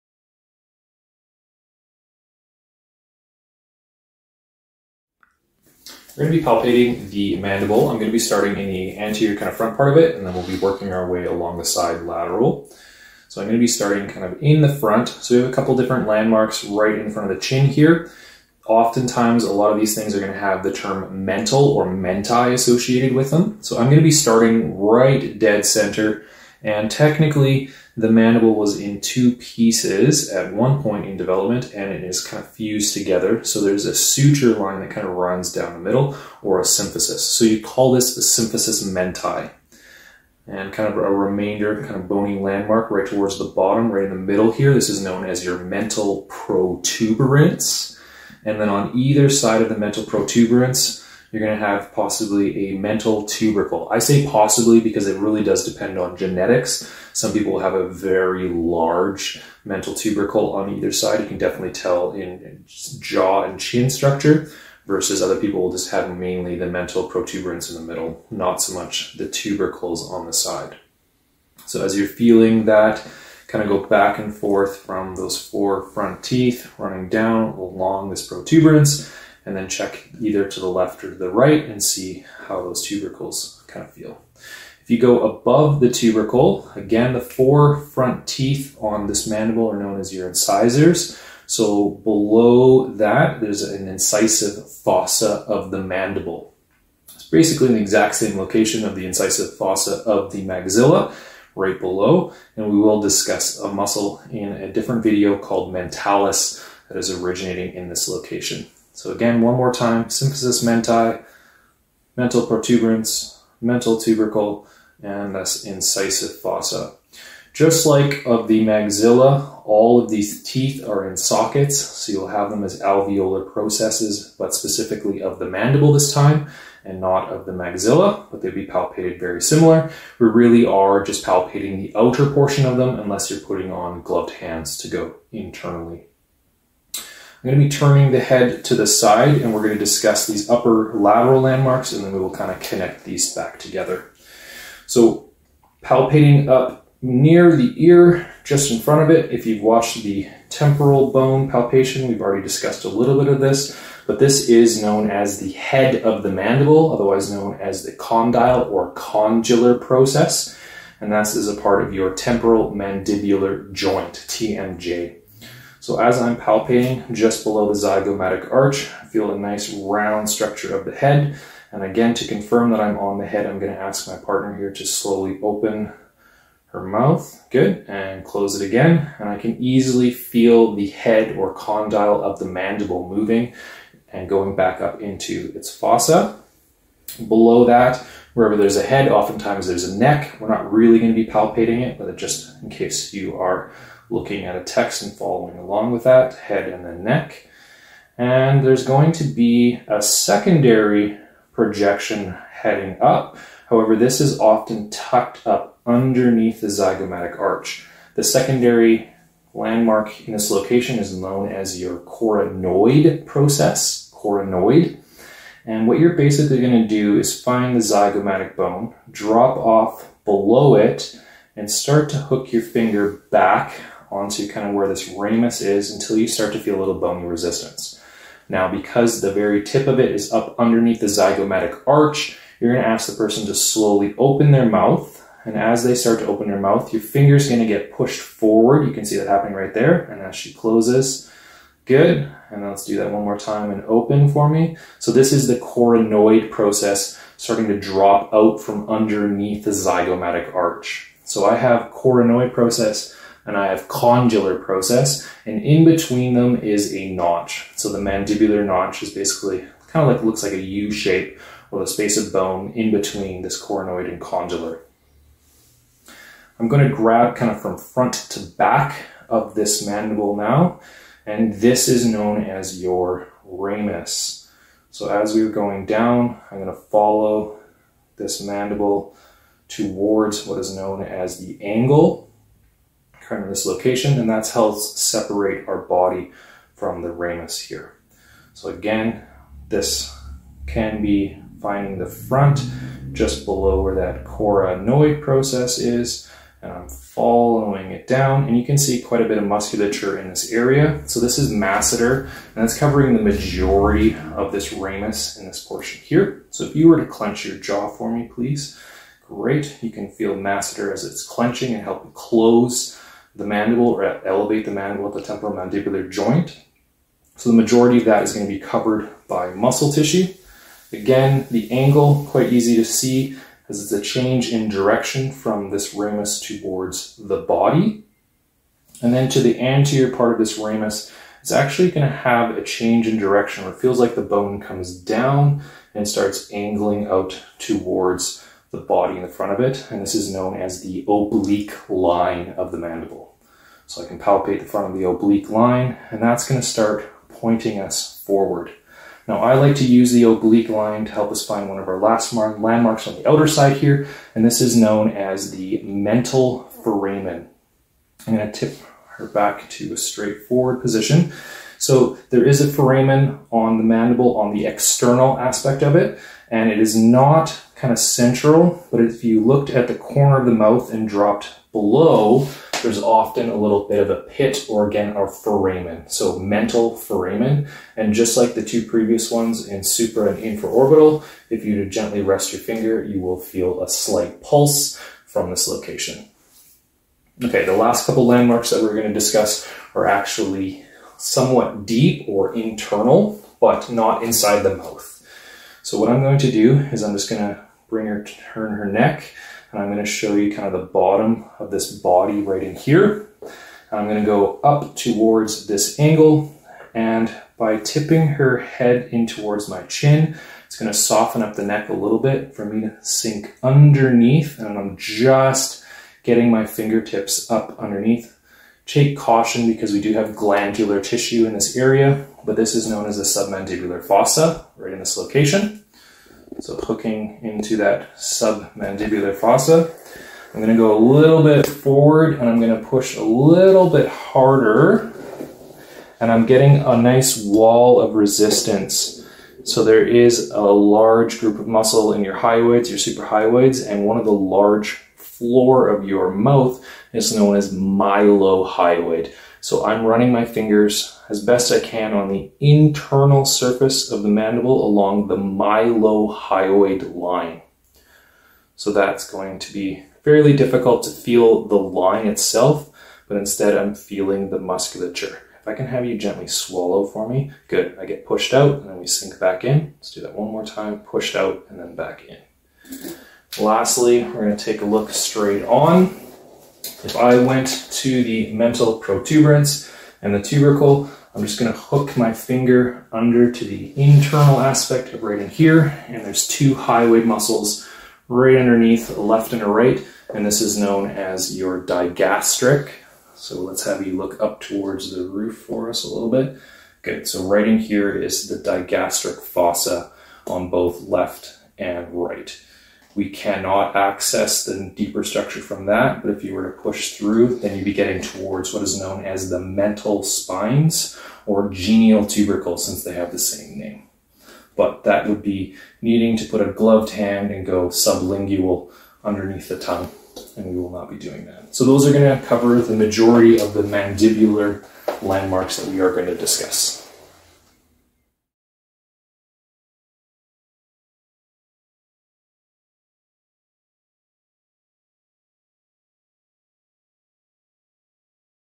Bony Landmark Palpation - Mandible (Skull)
This is a detailed palpation of bony landmarks of mandible bone of the skull by Keith Bootsma, RMT.

Model - Miah Thistle